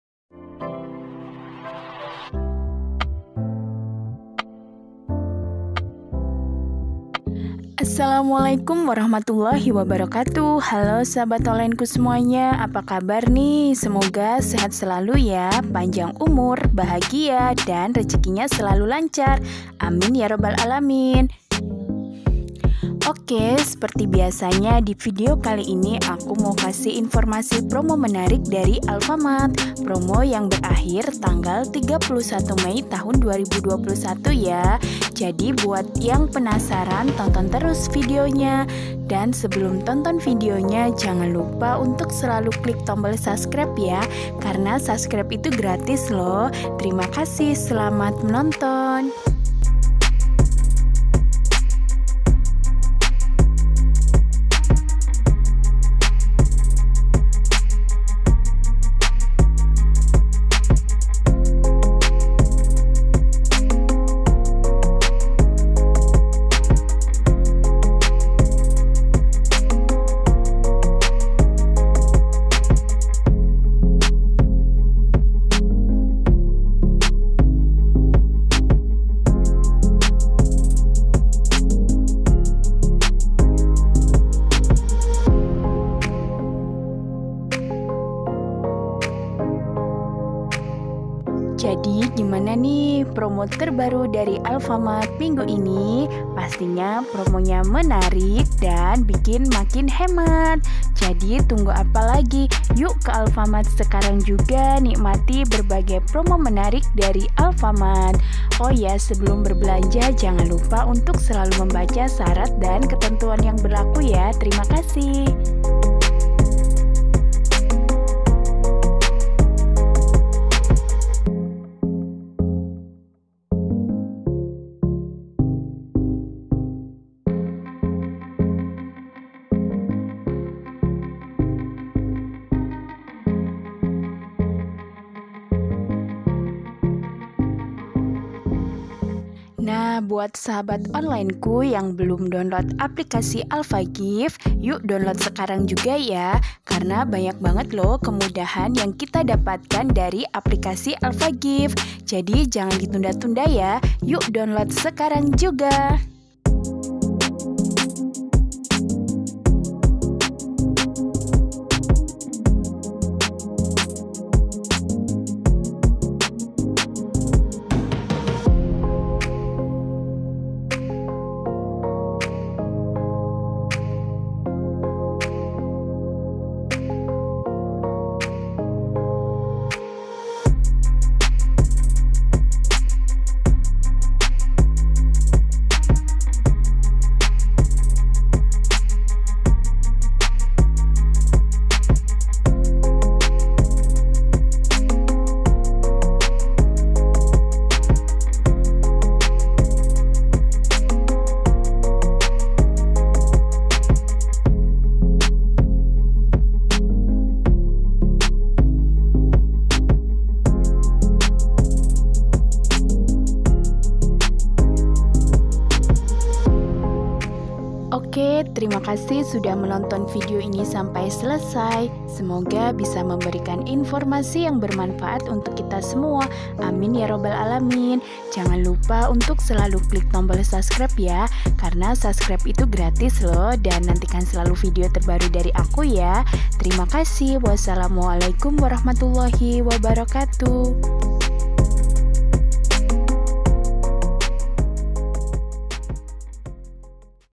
PROMO TERBARU ALFAMART HARI INI PERIODE TANGGAL 16-31 MEI 2021|PROMO CASHBACK 10% DARI HAJATAN GOPAY
Assalamualaikum wr.wb.
Hallo sahabat onlineku..
Kembali lagi di channel Ayu Erva Yulianto.
Di video kali ini, aku mau memberikan informasi mengenai promo menarik dari Alfamart periode tanggal 16-31 Mei 2021.. Jadi, pastinya Alfamart punya promo-promo terbaru dan sangat menarik dengan harga super spesial... Yuks penuhi kebutuhan harian Anda dengan nikmati promo terbaru yang buat belanja jadi hemat.. Nah, buat yang belum download aplikasi alfagift, yuks download sekarang juga ya, karena banyak keuntungan berbelanja yang bisa Anda dapatkan..
Berikut gambar katalog lengkap promo Alfamart terbaru..
Selamat menonton..

Terimakasih..

Wassalamualaikum wr.wb.